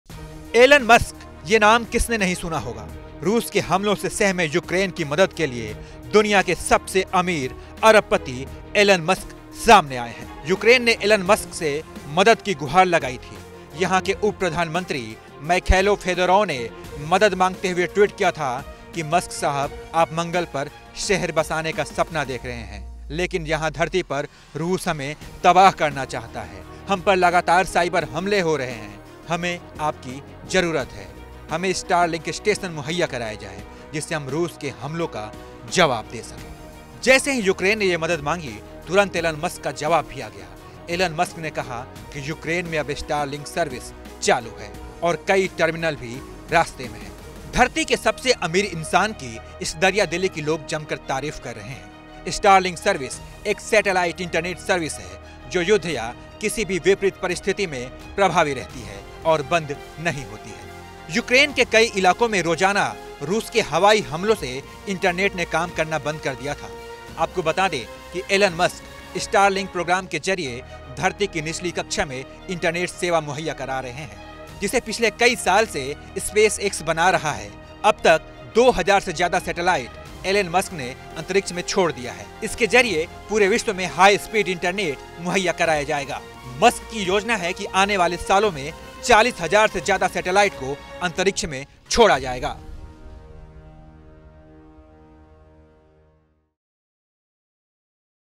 Russia-Ukraine : Elon Musk ने सुन ली Ukraine की गुहार, मदद की जमकर हो रही तारीफ
Elon Musk ने Ukraine की मदद की है. जिसकी लोग जमकर तारीफ कर रहे हैं. इस वीडियो में बताएंगे पूरी बात